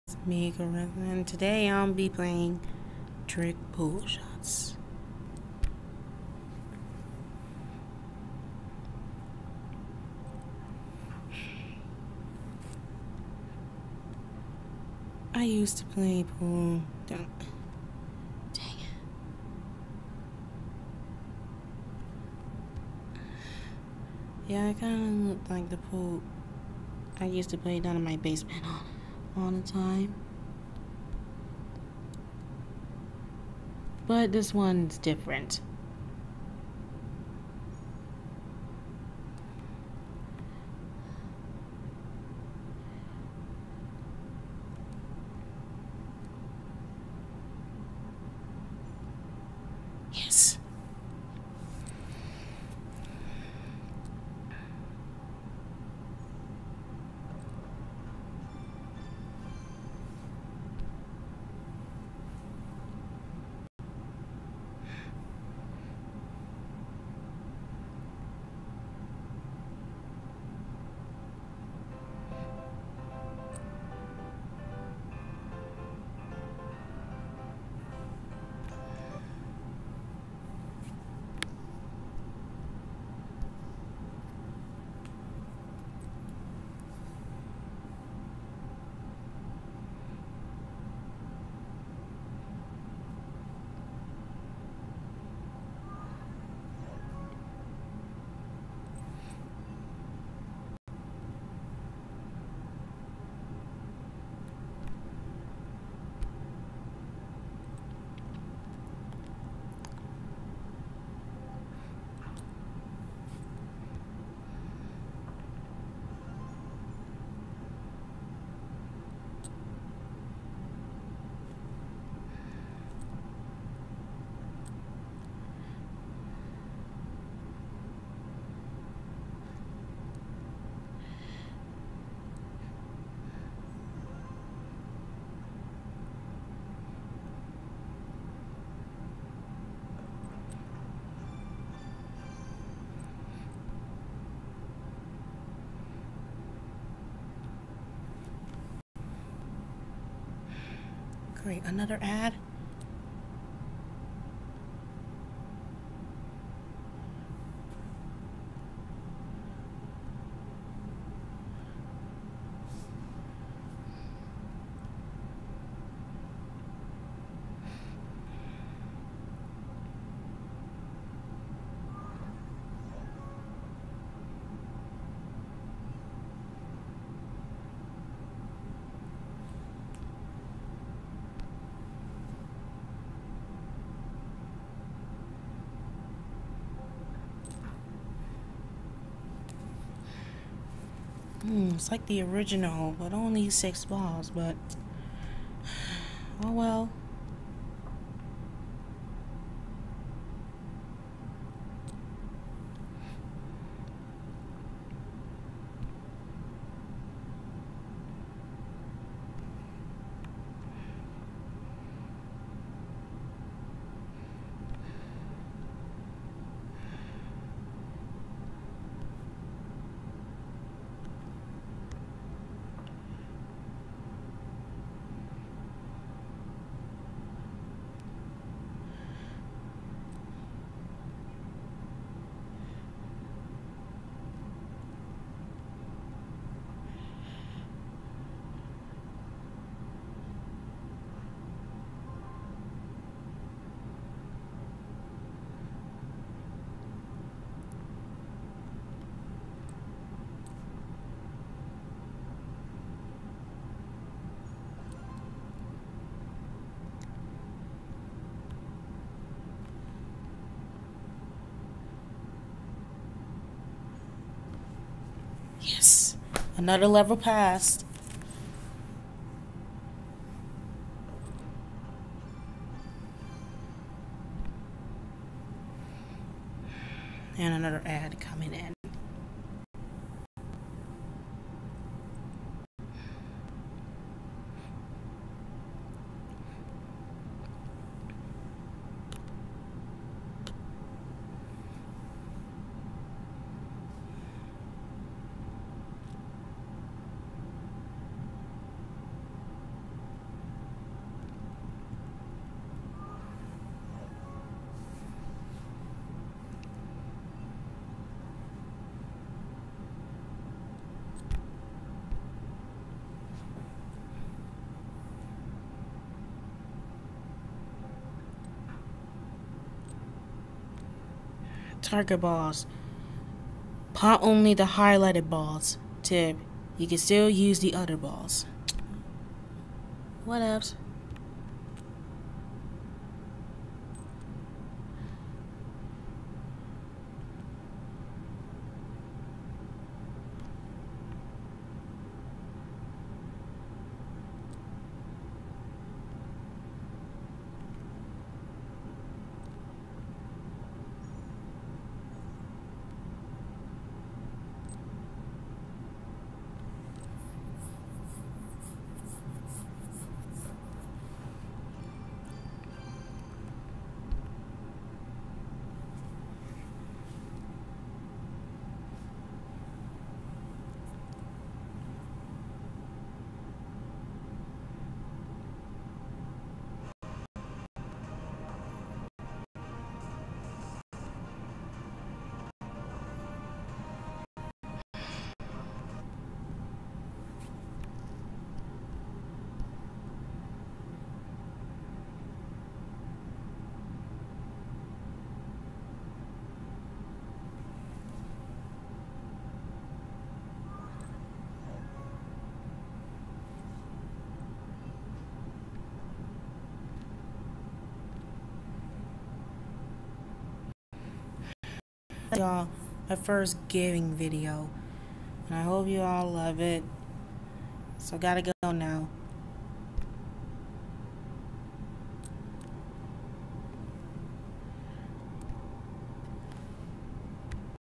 My 1st Gaming Video💻🎮📹: Pool Trickshots🎱
I know you guys are bored of my videos I've posted every Sunday so I decided to do a gaming video.